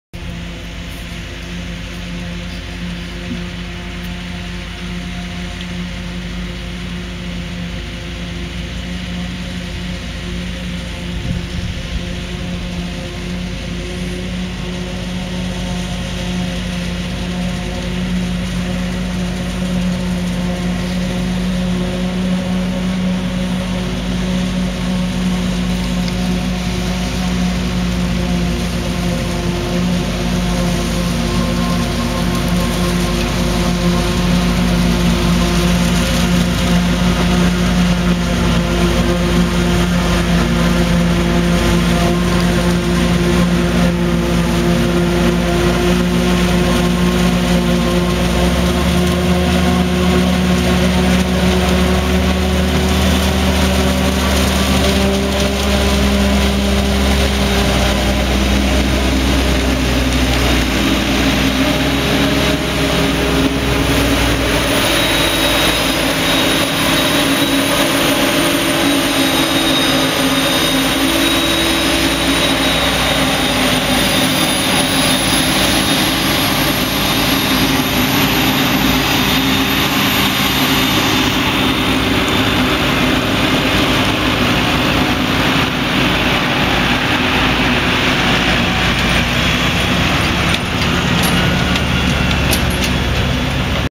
Ensilage 2022 claas jaguar 940 avec tracteur New Holland et Valtra
Vidéo 5 du 17 aout 2022
L'ensileuse avec un New Holland suivit par un Valtra.